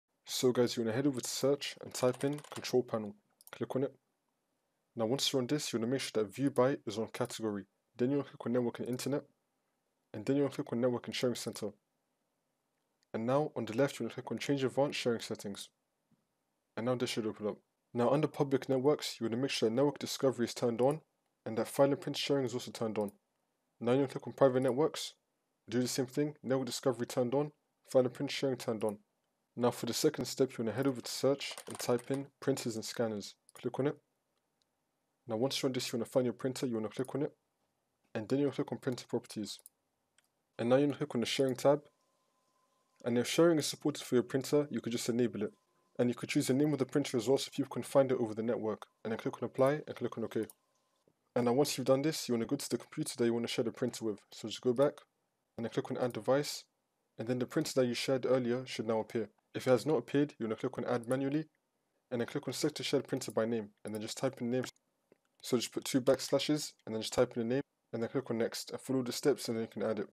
How To Share Printer on a Network in Windows 11/10
If you want to share your printer to multiple computers in Windows 11, you came to the right place. Connecting a printer to multiple computers on the same network gives you the advantage of printing to the same printer from all the computers on the network.

In Windows 11, as in Windows 10, you can share a printer connected to your computer (for example, via USB cable)* so that you can print from other computers on the network.

Knowing how to share a printer over your network on a Windows computer can be useful. The only option for users to print from the same device without printer sharing is for them to connect the printer to their computers. In this guide, we’ll show you how to share your printer on Windows 11 amongst your network PCs so you don’t have to connect a printer to each one individually.

When a printer is shared via a network, just one PC or laptop is required to be connected. Anyone on the same network can use the printer as long as it is connected, and they do not need to set it up on their own computer.